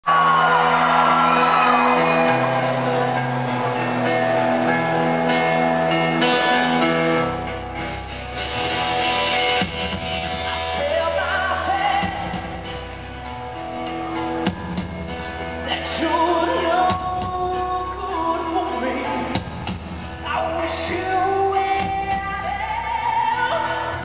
Halestorm (Uproar Festival)
Halestorm performing at Darien Lake Aug 22